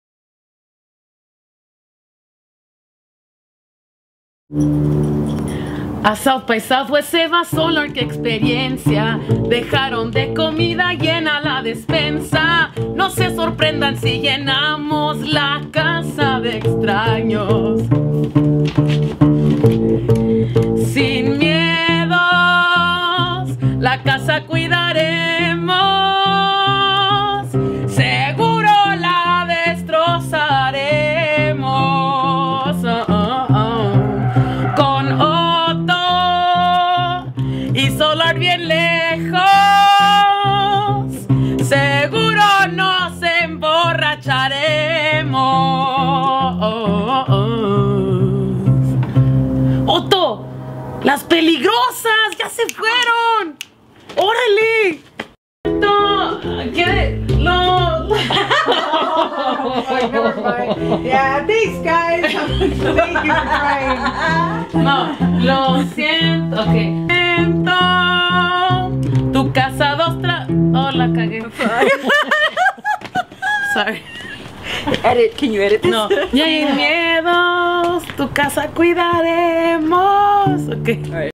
SOLART RADIO SXSW PROMO 2012.mov
SolArt Radio travels to SXSW this year to visit our dear friends and talented musicians Andrea Balency, Enjambre, Torreblanca as well as La Banda Elastica. Performing in this video are Marisoul and Gloria Estrada from LA's band La Santa Cecilia. March 2012. Recorded by Carla Zarate S. SolArt Radio, promoting Solidarity through ALL the arts.